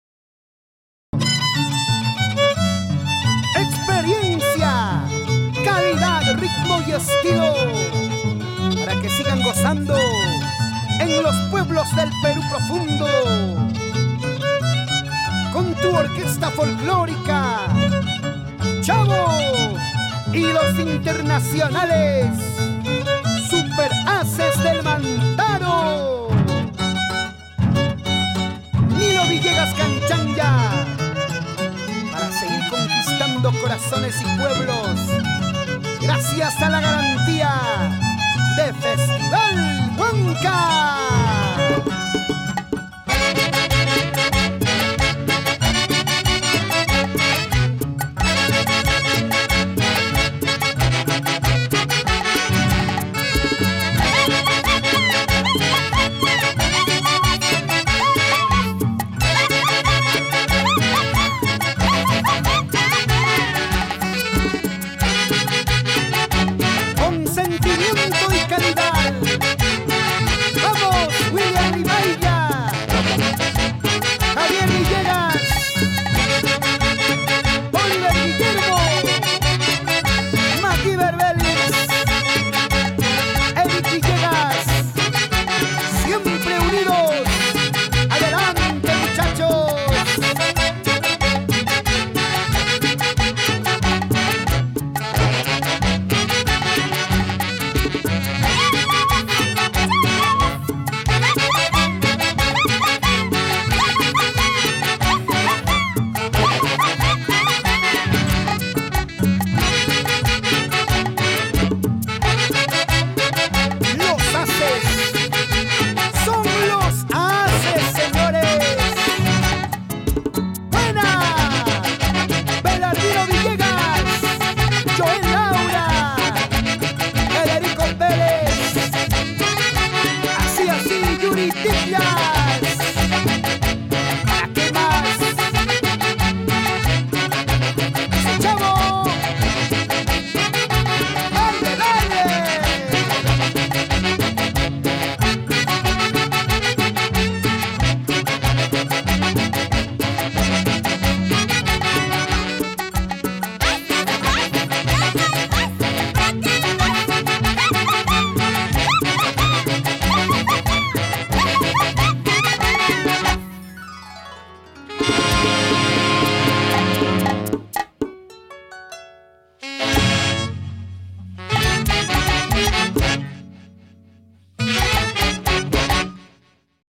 ORQUESTA FOLKL. CHAVO Y LOS SUPER ASES DEL MANTARO HUAYLARSH 2022 TEMA 08 CENTRO SOCIAL HUANCAYO
ORQUESTA FOLKLORICA  INTERNACIONAL CHAVO Y LOS SUPER ASES DEL MANTARO HUAYLARSH 2022 TEMA 08 CENTRO SOCIAL HUANCAYO NDE NILO VILLEGAS CANCHANYA